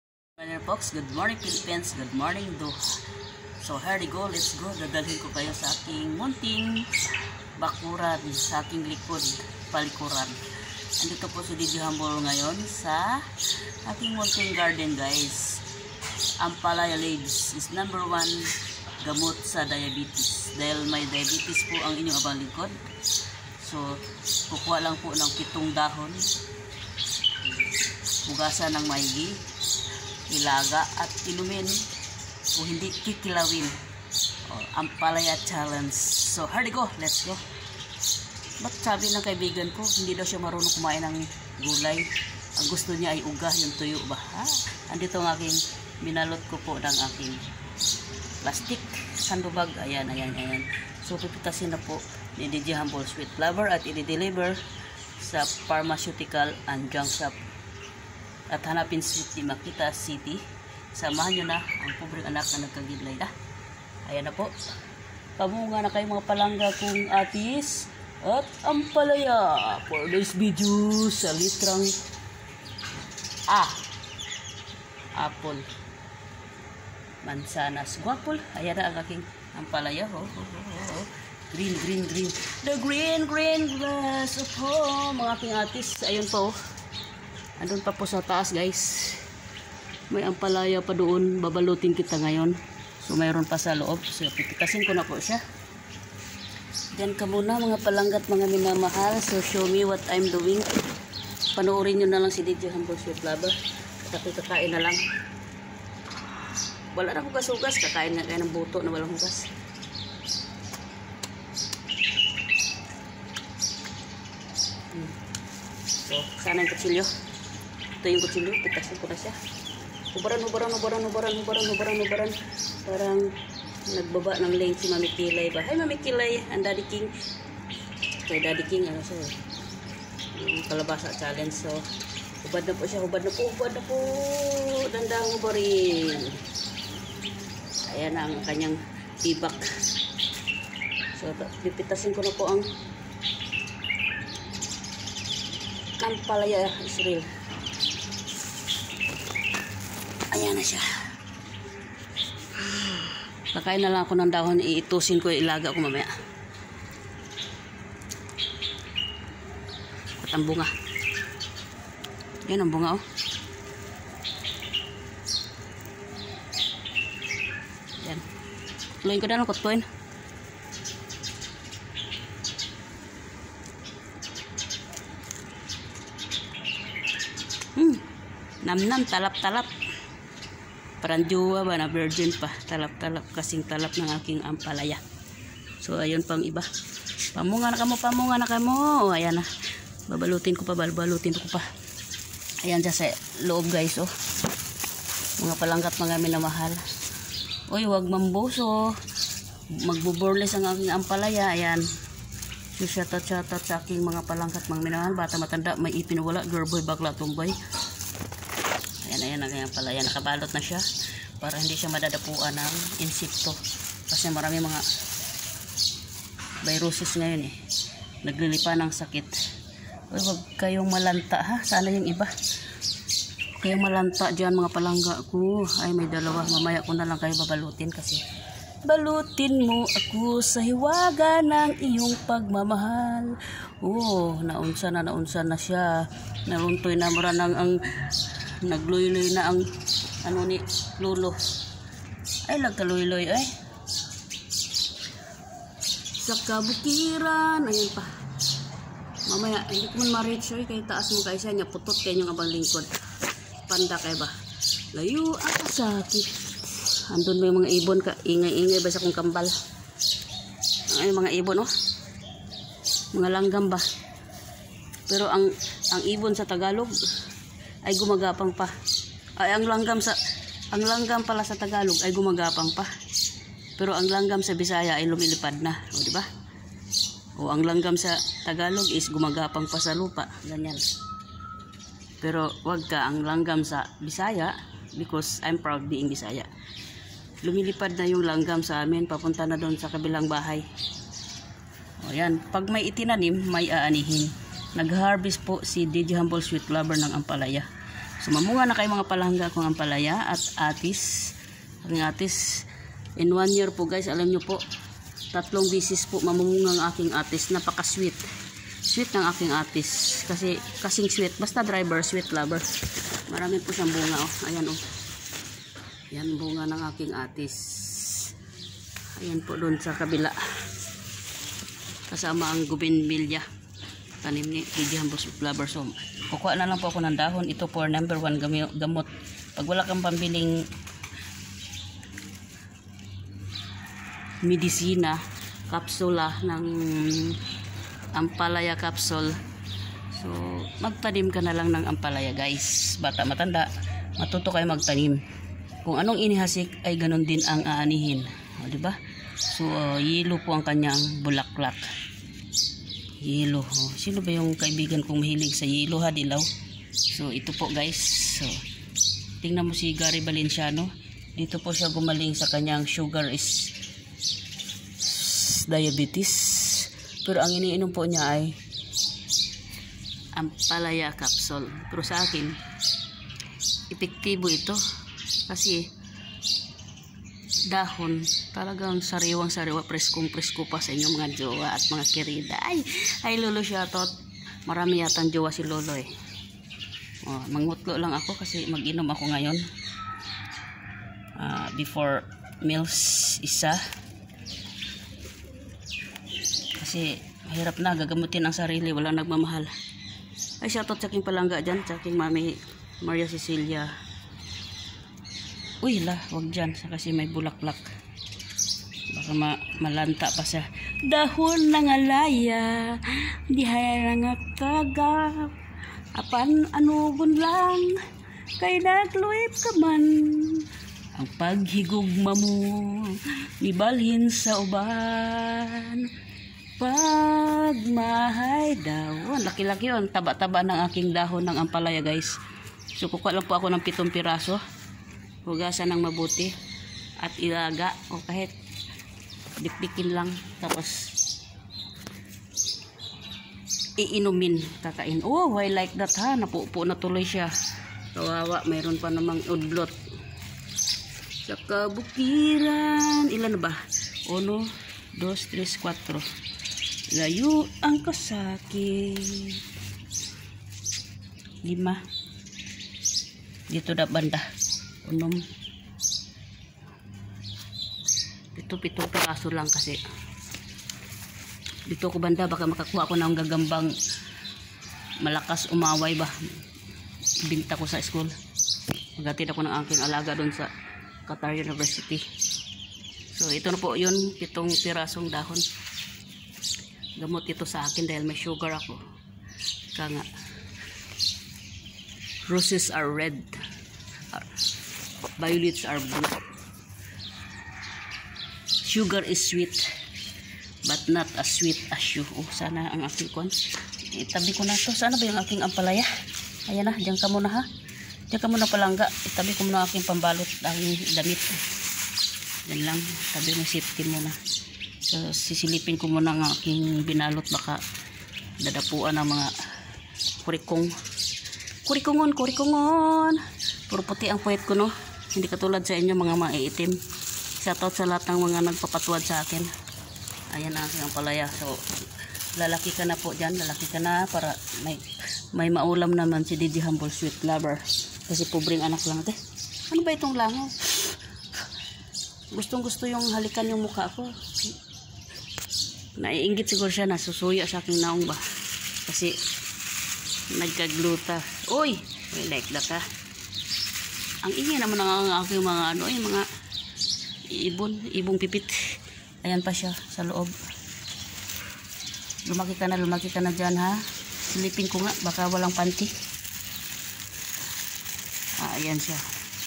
SA AKING HARDIN AMPLAYA CHALLEGE GAMOT SA DIABETICS
WALANG KWARTANG PANGBILI NG GAMOT KAYA KUMAIN NG DAHON AT BUNGA NG AMPLAYA GAMOT SA DIABITIS